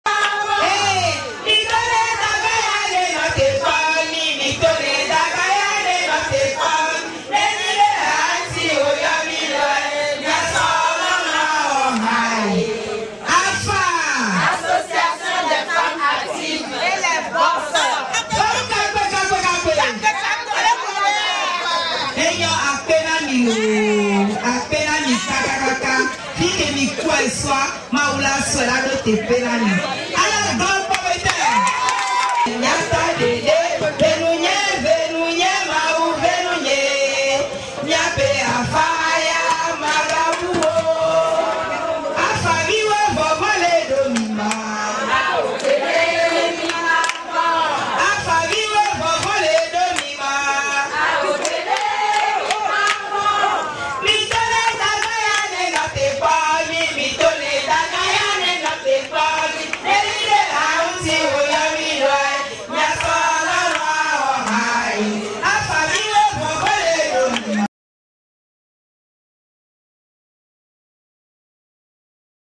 Ambiance des femmes de l'Association AFAB-CTE de Lomé /Togo .
Les obsèques de la veuve ANANIJosephine Woyomé née SEDEODJI dite Togoto ce 07 et 08 décembre 2018  à Agbavi au Togo.